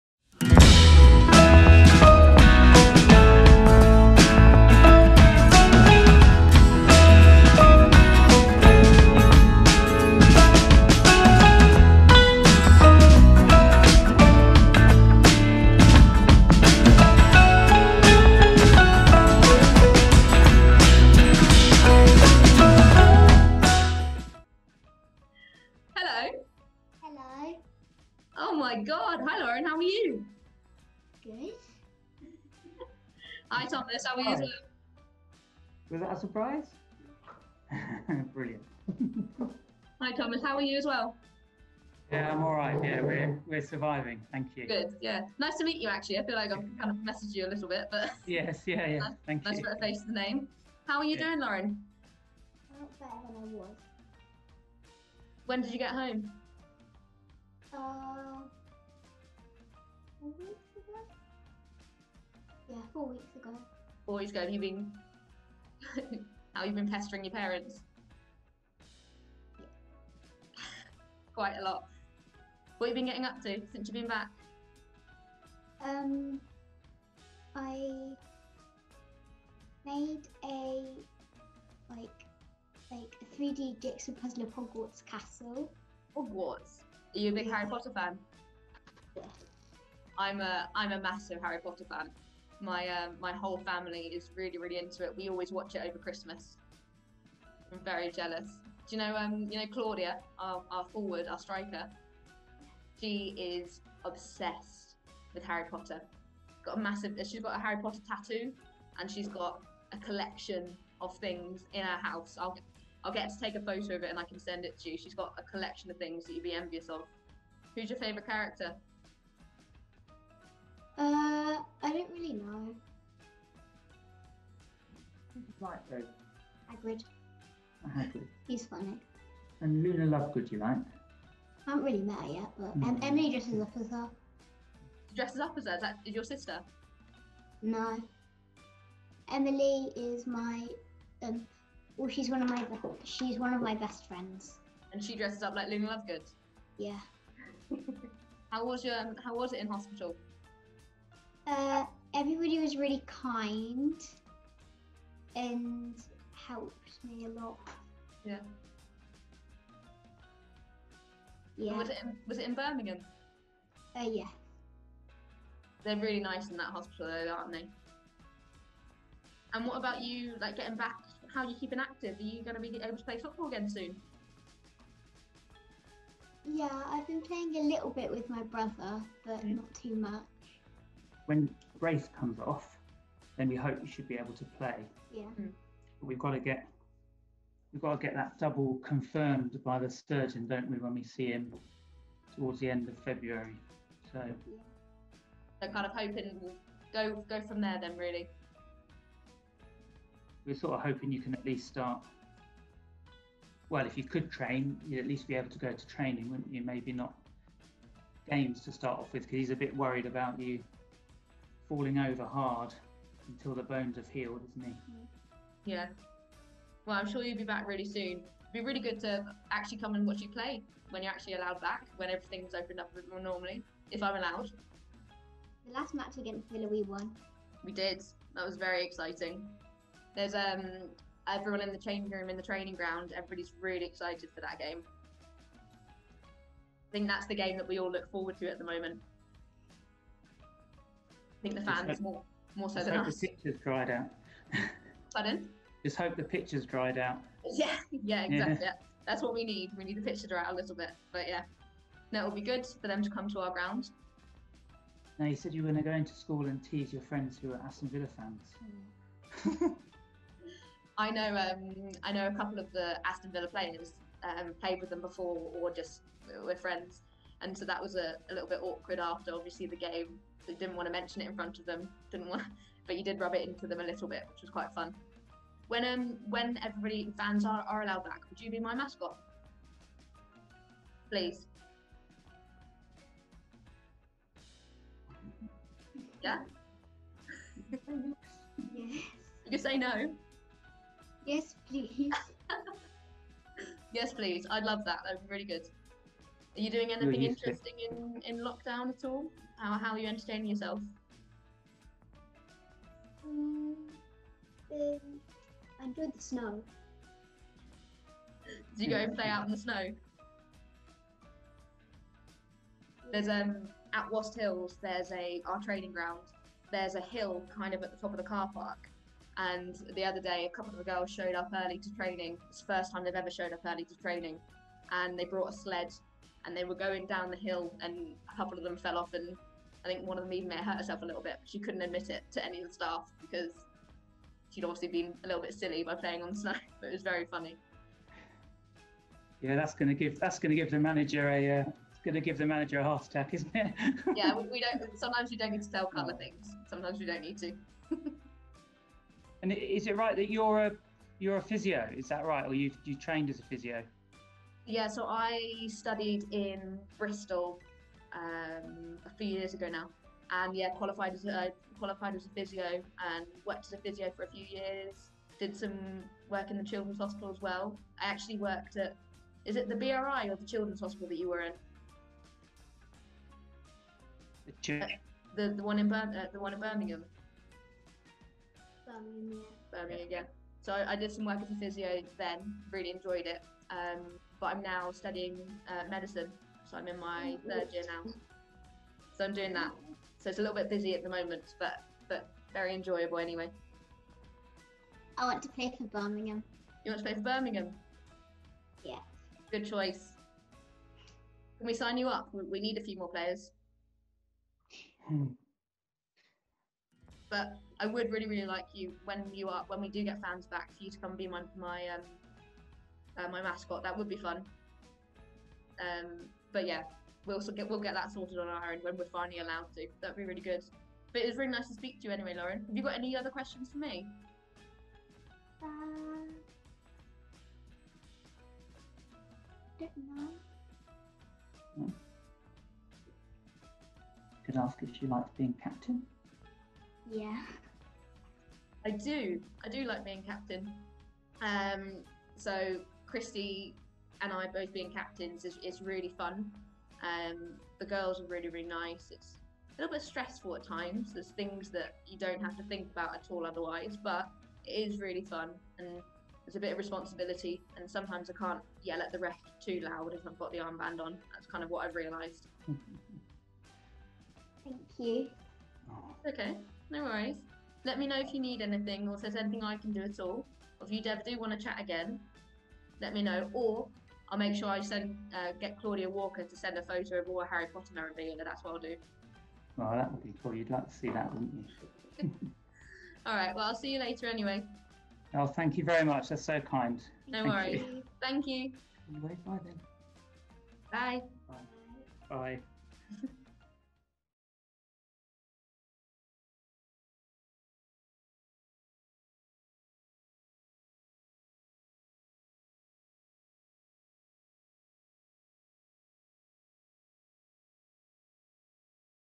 Lauren's surprise call with Harriet Scott
Club Captain, Harriet Scott, greeted Blues Women's youngest season ticket holder as she recovers from a spinal tumour.